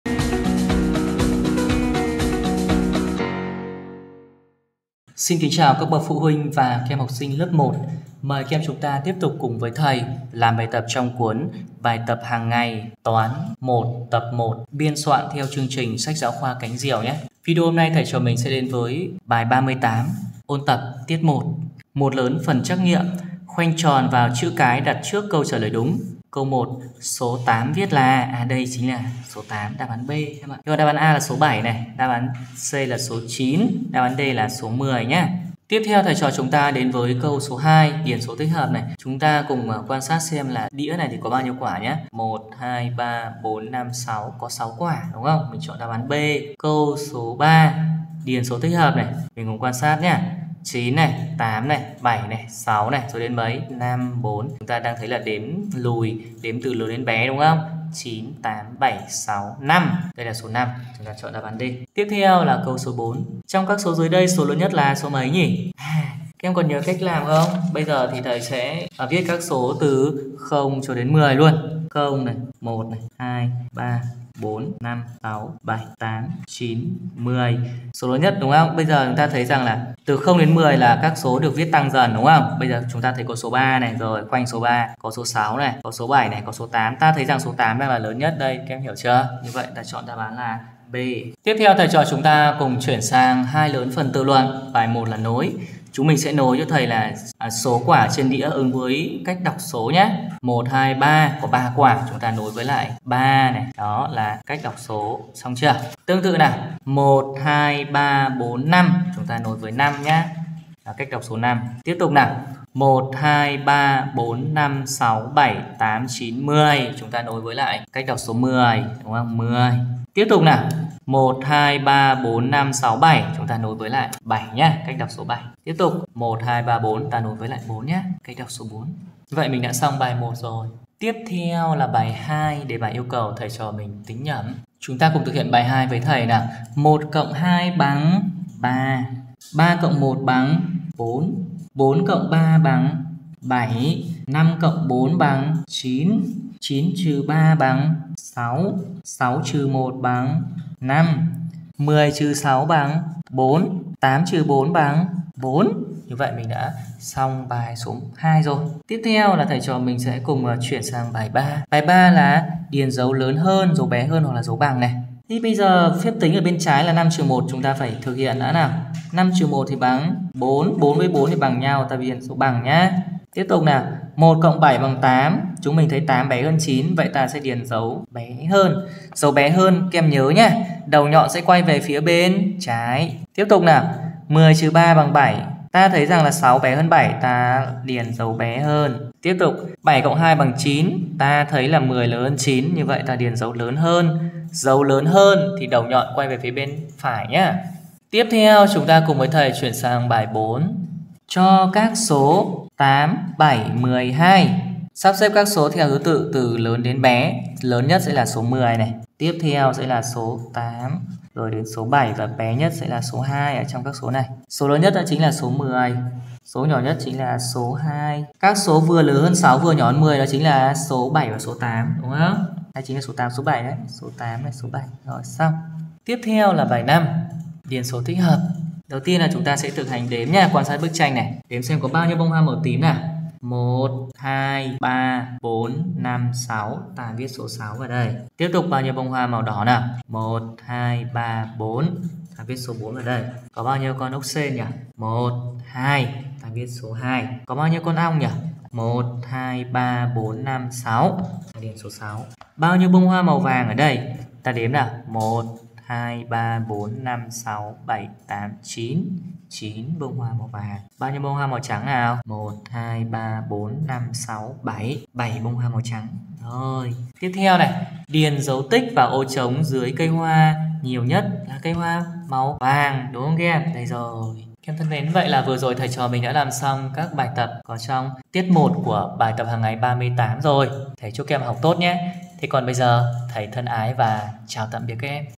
Bài 38 | Ôn tập (tiết 1) | Bài tập hằng ngày | Toán lớp 1 | Cánh diều | Thầy Nguyễn Văn quyền
Kính thưa quý phụ huynh và các em học sinh thân mến!

Thầy Quyền rất hân hạnh được giới thiệu với các bạn video "Bài 38 | Ôn tập (tiết 1) | Bài tập hằng ngày | Toán lớp 1 | Cánh diều | Thầy Nguyễn Văn quyền" do Thầy Nguyễn Văn Quyền trực tiếp biên soạn.

Là một phần của chuỗi bài giảng môn toán, Thầy Nguyễn Văn Quyền đã khéo léo kết hợp các khái niệm toán học với cuộc sống hàng ngày để mang lại trải nghiệm bổ ích và hấp dẫn cho các em học sinh lớp 1. Trong video này, Thầy Nguyễn Văn Quyền hướng dẫn từng bước làm các bài tập toán lớp 1 về nội dung ôn tập tiết 1 lại các nội dung toán đã học trong sách bài tập hằng ngày toán 1 Cánh Diều.

Các bài tập được trình bày trong video dựa trên kiến thức và kỹ năng được xây dựng trong sách giáo khoa cánh diều, qua đó giúp các em củng cố kiến thức đã học và nâng cao khả năng giải toán

Thầy mong rằng, các bậc phụ huynh và các em học sinh xem kỹ video của Thầy Nguyễn Văn Quyền để nắm vững các khái niệm, rèn luyện kỹ năng và học tập xuất sắc môn toán lớp 1.